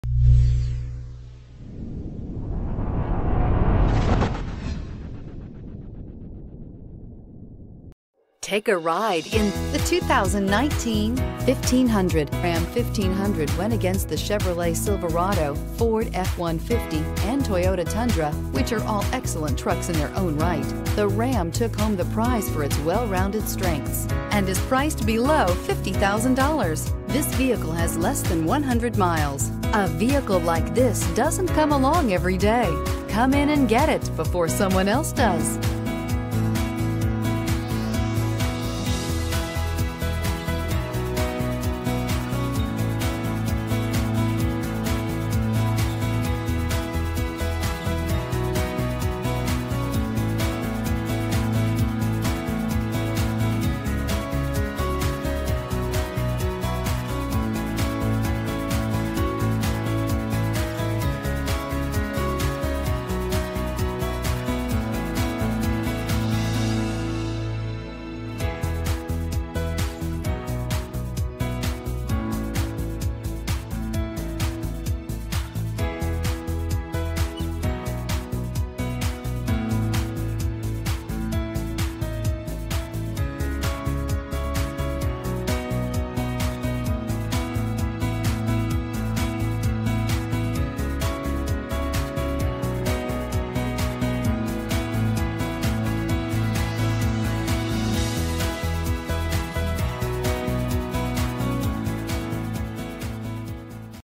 2019 Ram 1500 Midland TX KN562767
2019 Ram 1500 Big Horn 4x2 Crew Cab 5'7 Box

This Ram won't be on the lot long! Without a doubt, this is a technologically advanced vehicle for the sport-inclined driving enthusiast! It includes heated seats, an outside temperature display, adjustable pedals, and remote keyless entry. It features an automatic transmission, rear-wheel drive, and a powerful 8 cylinder engine. We have the vehicle you've been searching for at a price you can afford. Please don't hesitate to give us a call.